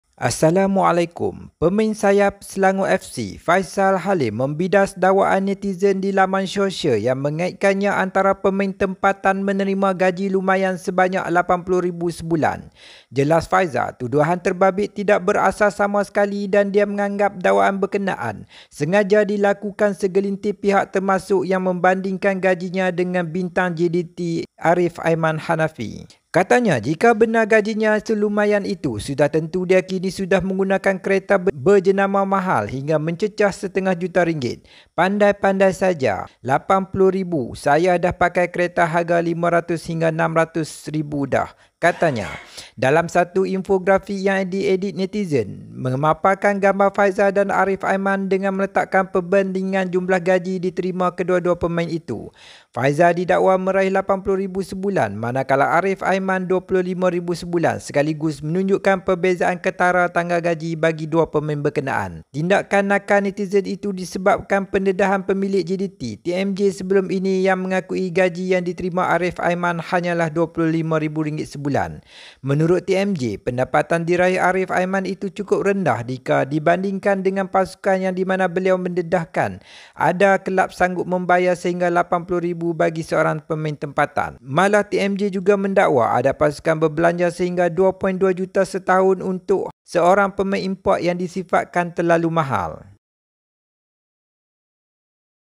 Ini Kata Faisal Halim Tentang Gajinya Di Selangor FC vs Arif Aiman Dari JDT
Pemain Selangor FC Faisal Halim menyatakan tentang info salah yang tular tentang Gajinya dan Arif Aiman dari JDT

PENAFIAN!!
"Penggunaan Gambar Dan Visual hanyalah Naratif Penceritaan Video"
Ia Milik mereka yang berkenaan dan saya berikan kredit kepada mereka.